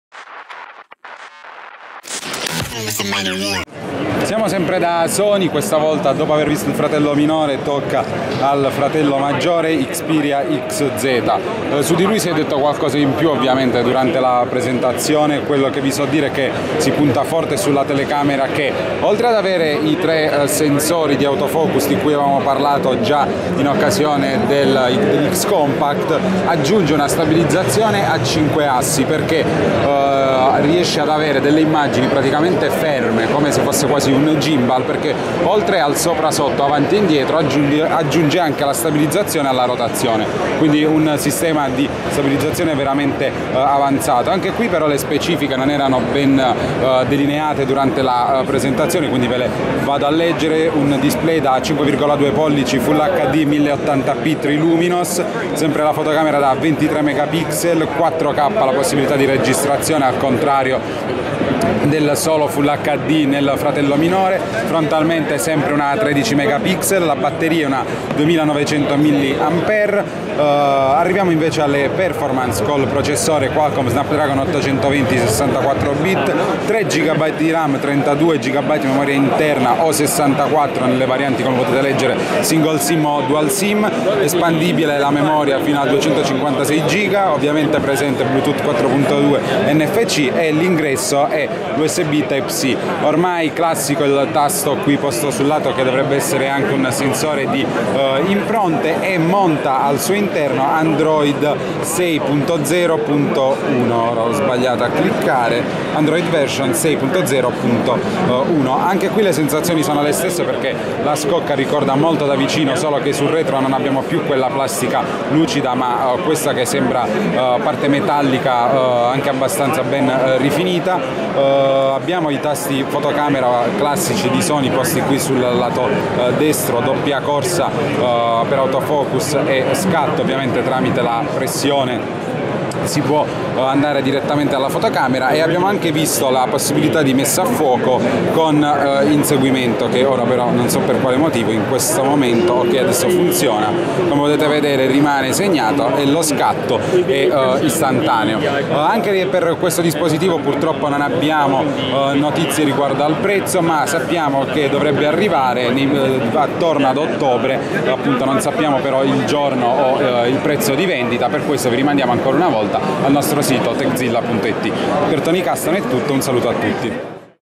Sony Xperia XZ - Hands on IFA 2106
Ecco il nuovo top di gamma Sony: fotocamera da 23 megapixel con una stabilizzazione a 5 assi e 3 sensori per l'autofocus e l'esposizione per un comparto fotografico che promette scintille!